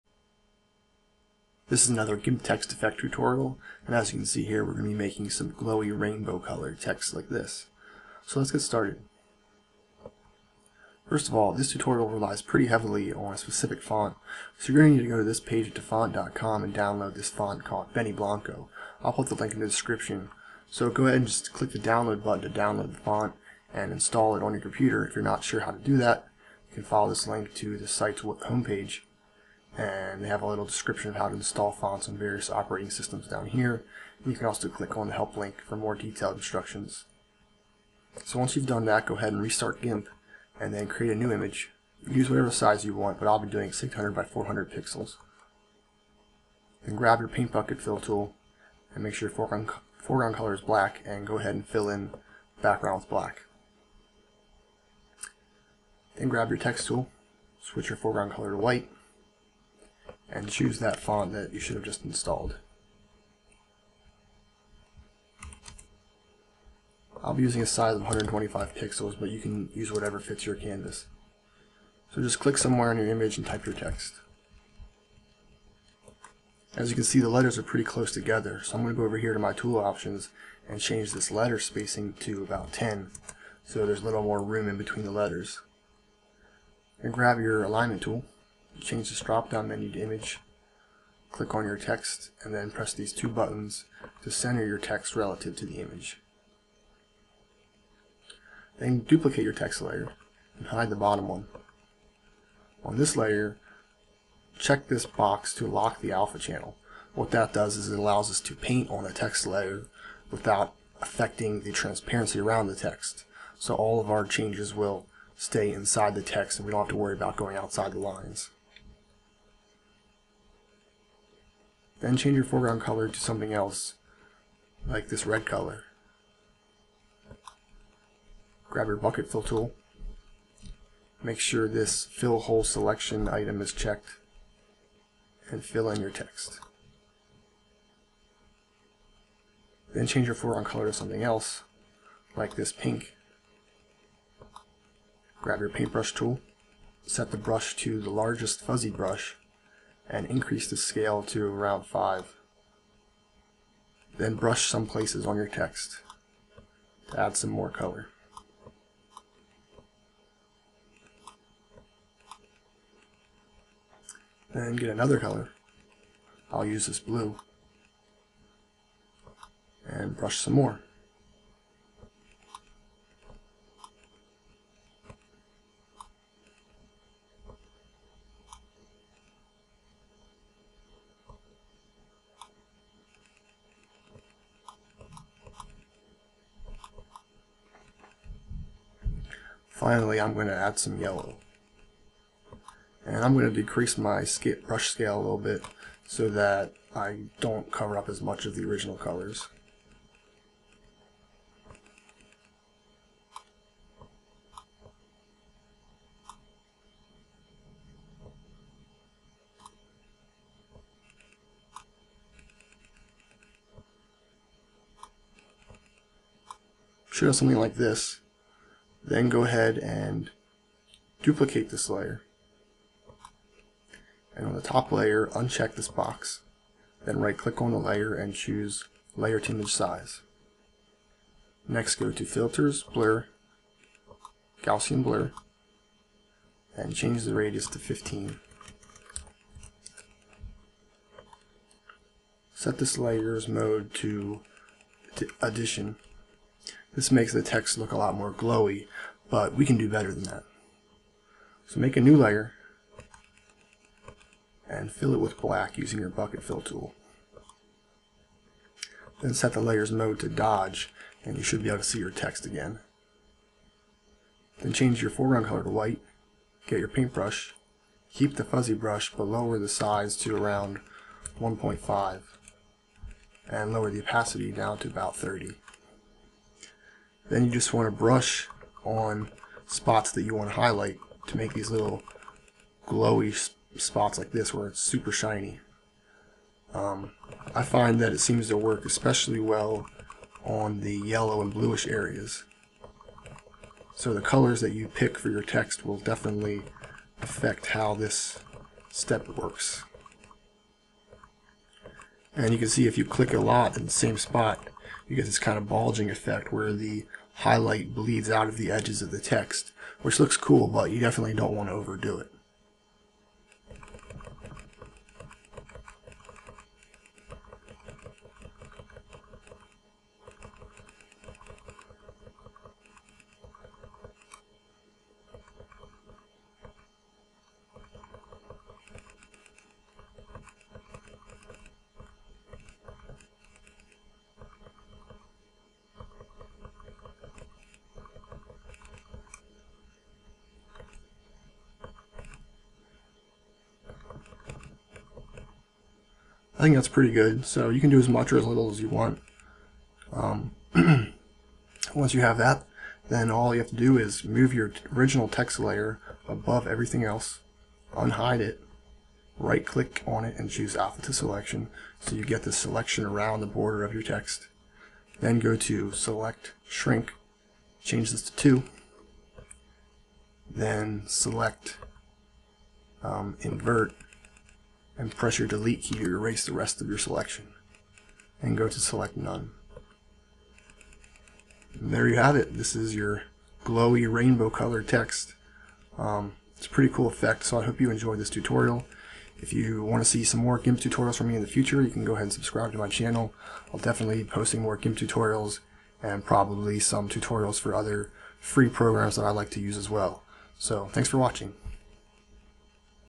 GIMP Tutorial: Super Glowy Rainbow Text Effect
GIMP is an open source (free) image editing program with many of the same features as Adobe Photoshop.